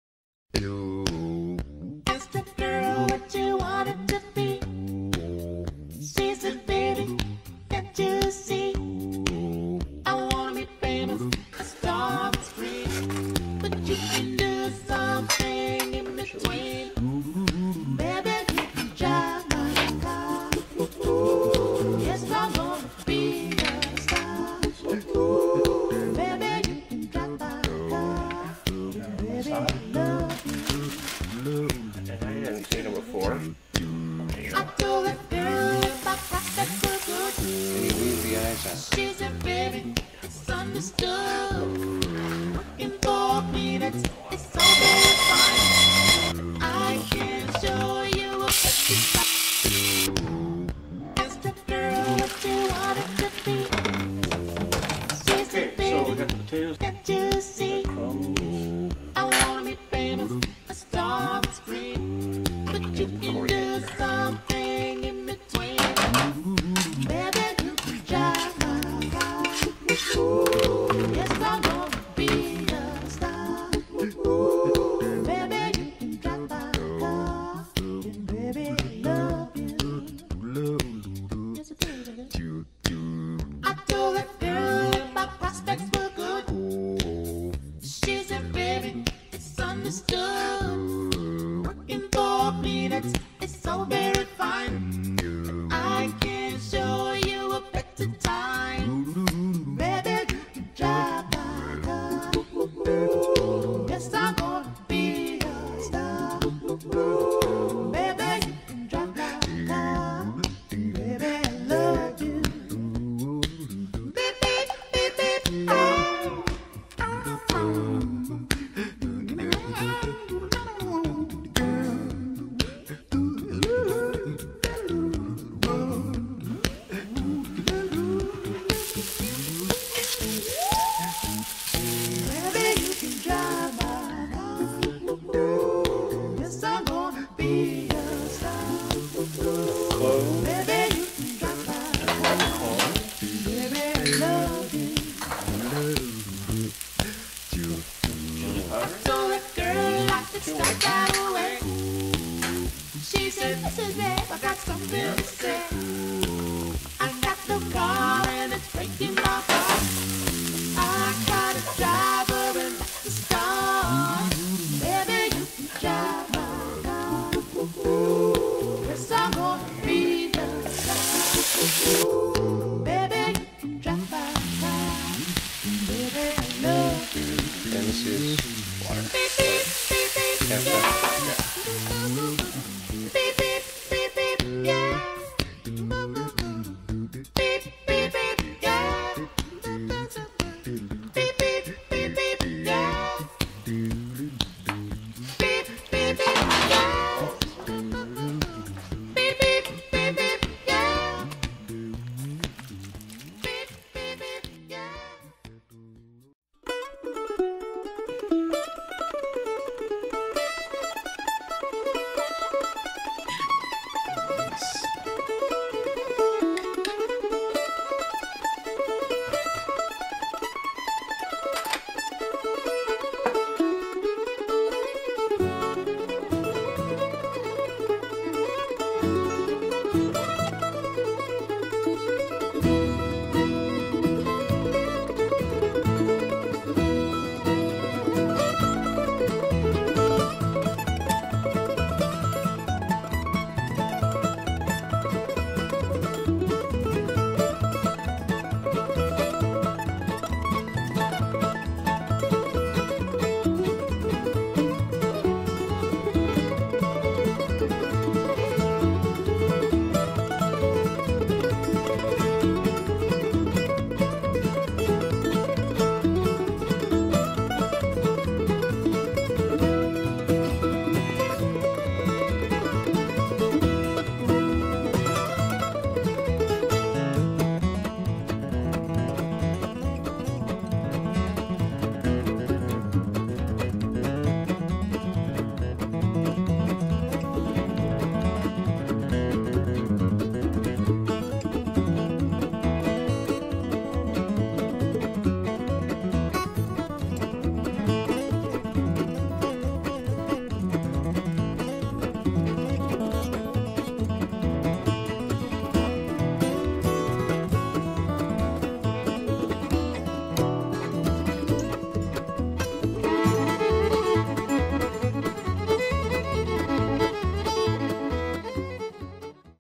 Fried Potato Balls
With a cream cheese center, these potato balls will blow you away!